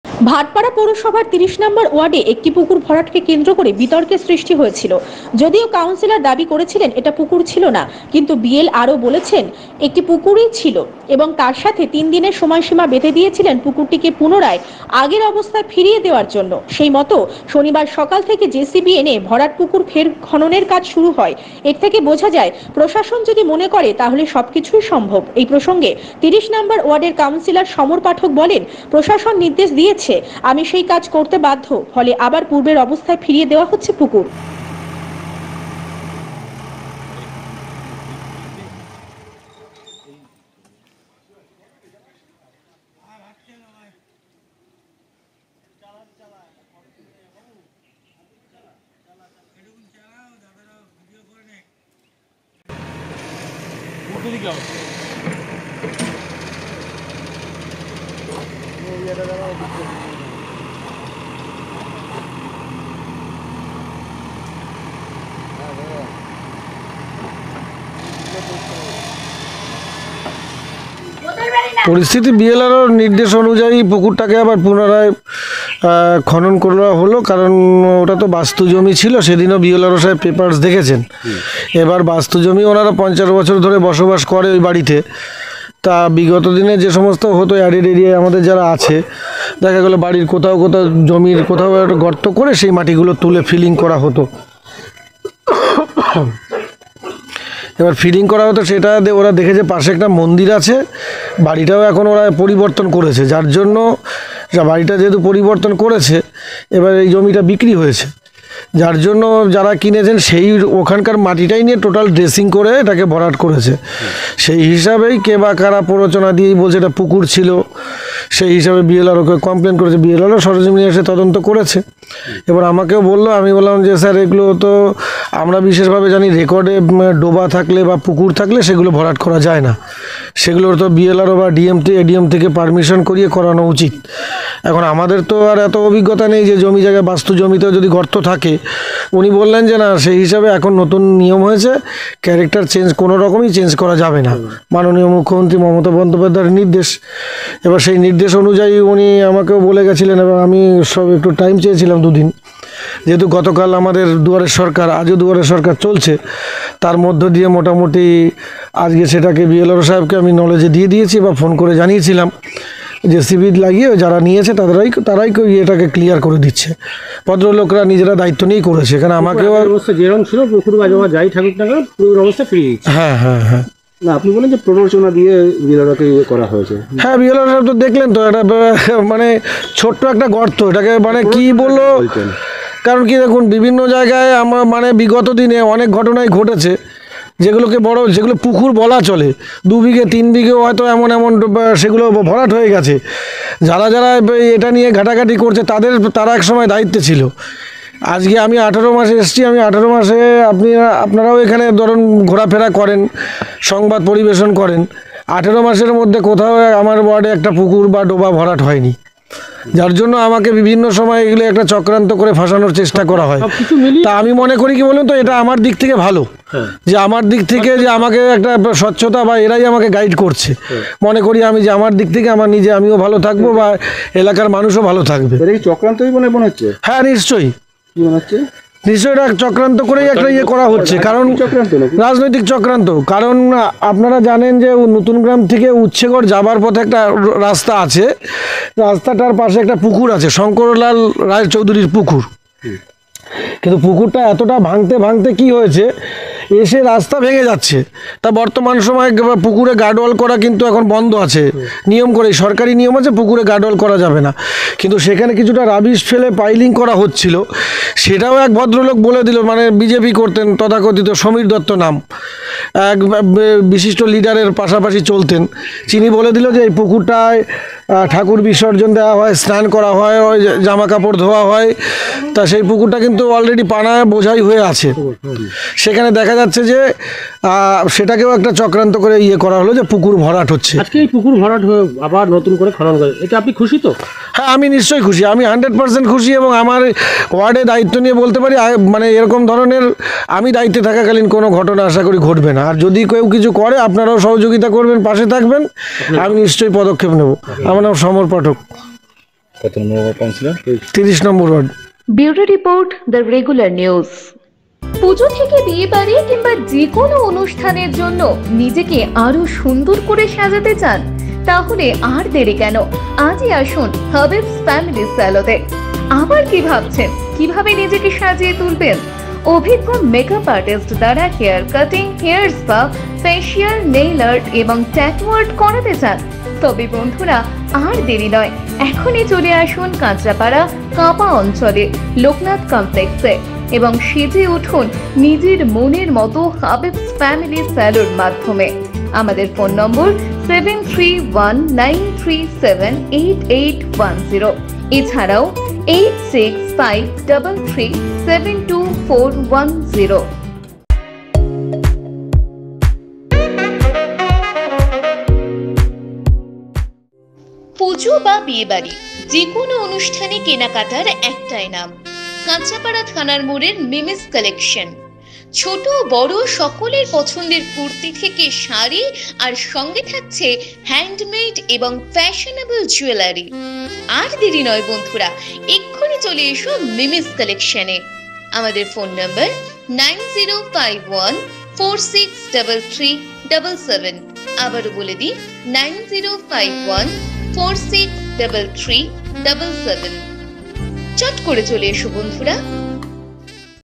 LIVE | ভাটপাড়ায় সরকারি নির্দেশে ভরাট পুকুর পুনরায় খননের কাজ শুরু
সমস্ত নিউজ আপডেট জানার জন্য চোখ রাখুন theregular.in এর পেজে | Get news update on theregular.in page | 17 Sep 2023

ভাটপাড়া পৌরসভার ৩০ নম্বর ওয়ার্ডে একটি পুকুর ভরাটকে কেন্দ্র করে বিতর্কের সৃষ্টি হয়েছিল। যদিও কাউন্সিলর দাবি করেছিলেন, এটা পুকুর ছিল না। কিন্তু বিএলআরও বলছেন, এটি পুকুরই ছিল এবং তার সাথেই তিন দিনের সময়সীমা বেঁধে দিয়েছিলেন পুকুরটিকে পুনরায় আগের অবস্থায় ফিরিয়ে দেওয়ার জন্য। সেই মতো শনিবার সকাল থেকে জেসিবি এনে ভরাট পুকুর ফের খননের কাজ শুরু হয়। এর থেকে বোঝা যায়, প্রশাসন যদি মনে করে তাহলে সবকিছুই সম্ভব। এই প্রসঙ্গে ৩০ নম্বর ওয়ার্ডের কাউন্সিলর সমর পাঠক বলেন, প্রশাসন নির্দেশ দিয়েছে, আমি সেই কাজ করতে বাধ্য। ফলে আবার পূর্বের অবস্থায় ফিরিয়ে দেওয়া হচ্ছে পুকুর!